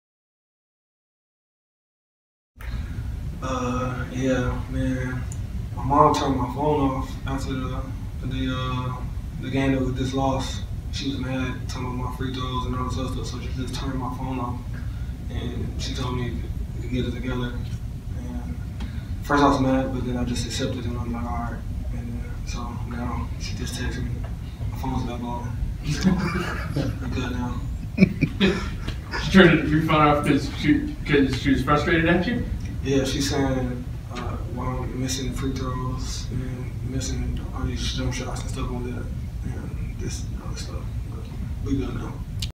Trey Davis: 'My Mom Turned My Phone Off'
The senior point guard explains why he was so motived in his 40-point game against New Orleans.